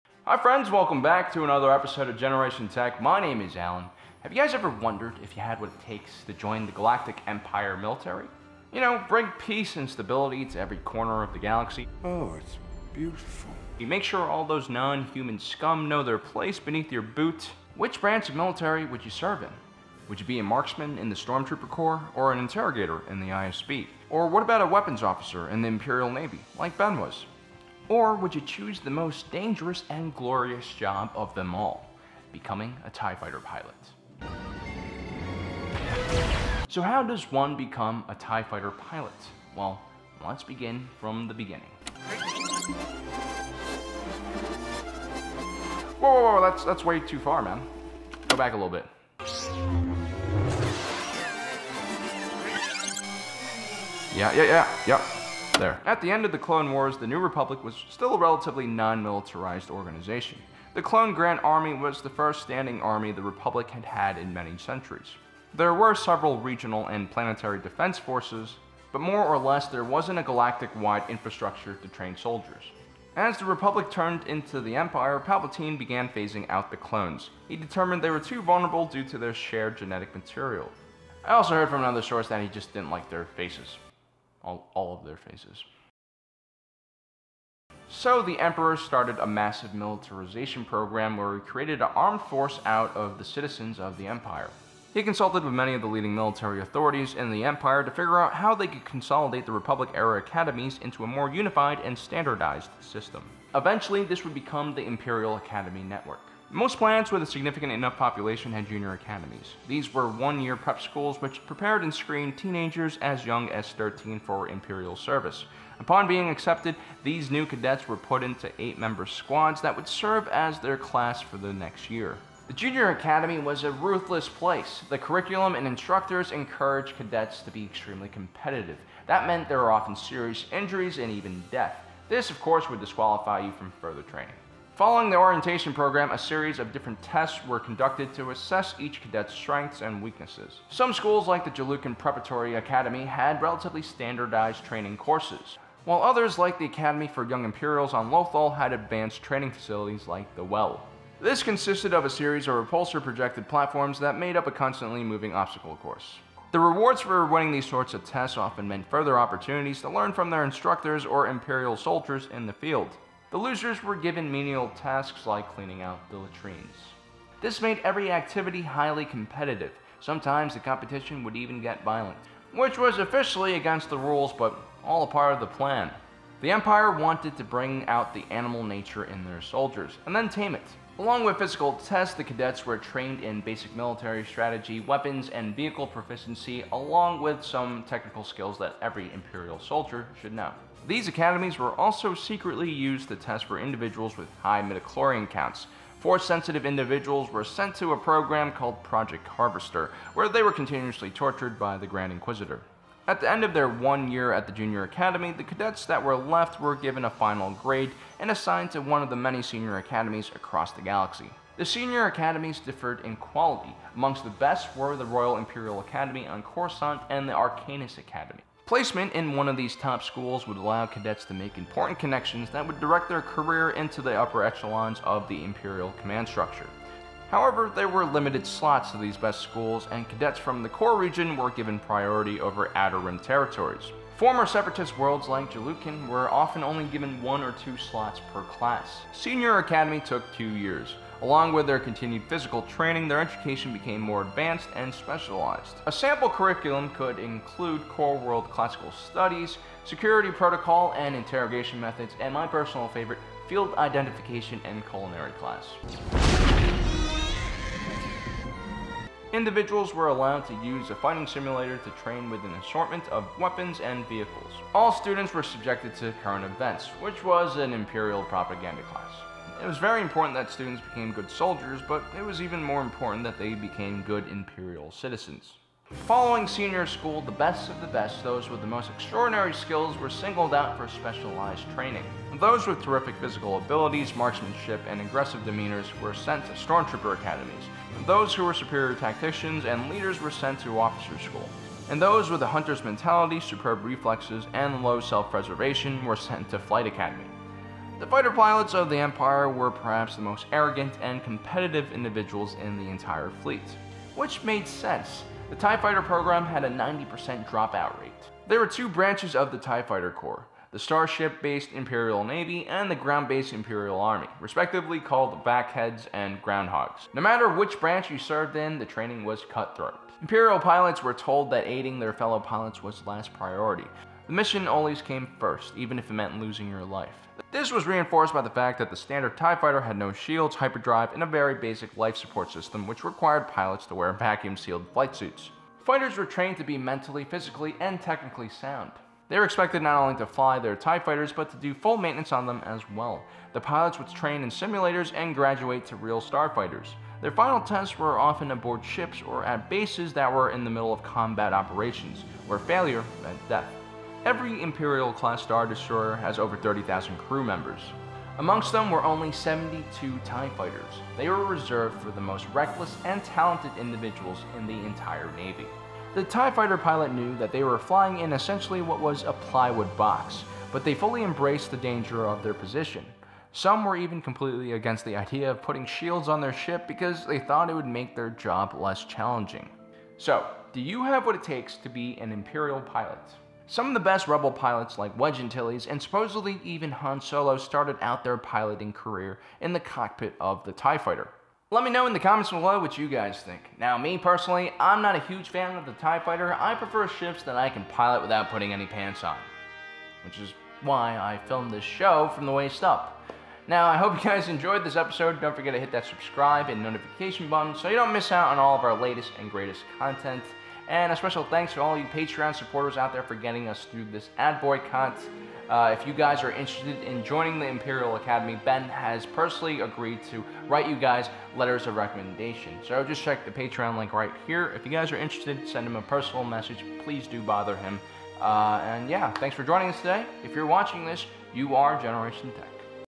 How to Become an IMPERIAL TIE FIGHTER PILOT | Star Wars Explained
How does one become an Imperial Navy Tie Fighter Pilot? We cover the very complex galactic empire recruiting and training process, from junior academy to your first combat mission. Another episode of star wars explained.

EQUIPMENT USED
CAMERA: SONY A6300
LENS: MITAKON 35MM F0.95
EDITING SOFTWARE